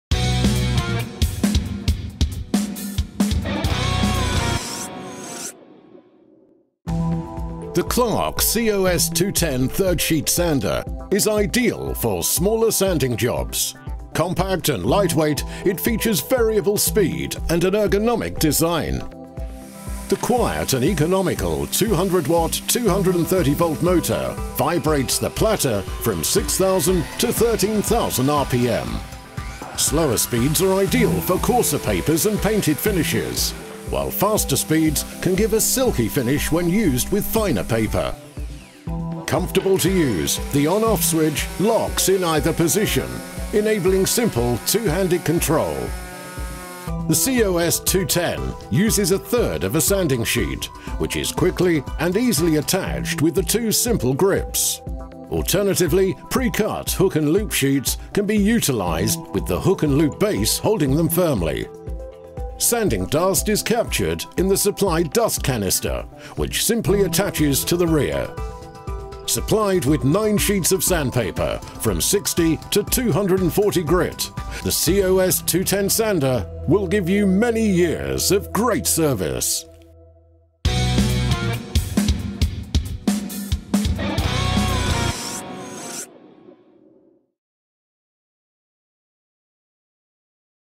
Clarke COS210 1/3 Sheet Sander - Clip-on and Hook & Loop - 230V
1/3 Sheet sander for smaller areas and sanding applications. Excellent as a first purchase to kick off your Clarke collection, for those restoration or DIY projects.